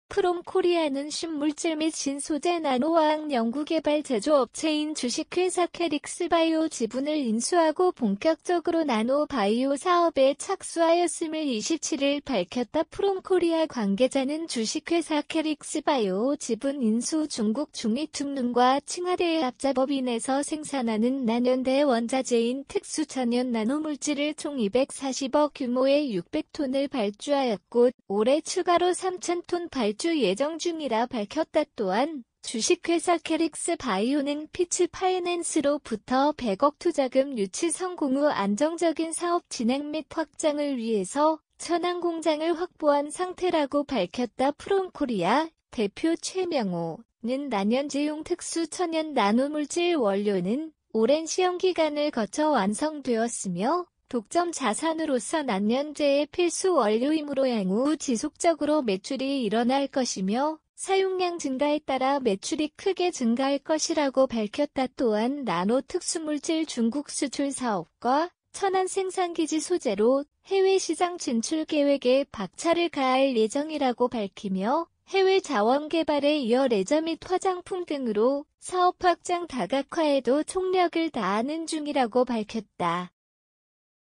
프롬코리아 , 나노기술 &바이오사업
프롬코리아는 신물질및 신소재 나노화학 연구개발 제조 업체인 (주)케릭스바이오 지분을 인수하고 본격적으로 나노 바이오사업에 착수하였음을 27일 밝혔다프롬코리아 관계자는 (주)캐릭스바이오 지분 인수후 중국 중휘퉁룽과 칭하대의 합자법인에서 생산하는 난연대원자재인 특수천연 나노물질을 총 240억 규모의 600톤을 발주하였고 올해 추가로 3000톤 발주 예정중이라 밝혔다또한 (주)캐릭스바이오는 피치파이낸스로 ...

김종현 기자